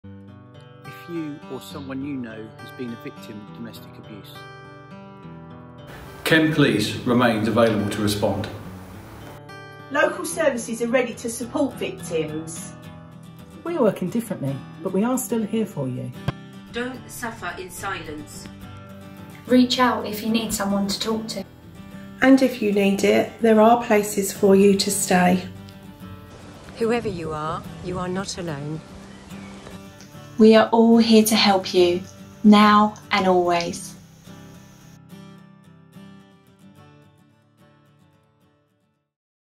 We are all here to help you - now and always
If you, or someone you know, has been a victim of domesticabuse, we are all here to help you - now and always.

Get help by calling @VS_Kent on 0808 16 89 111. Dial 999 if it is an emergency.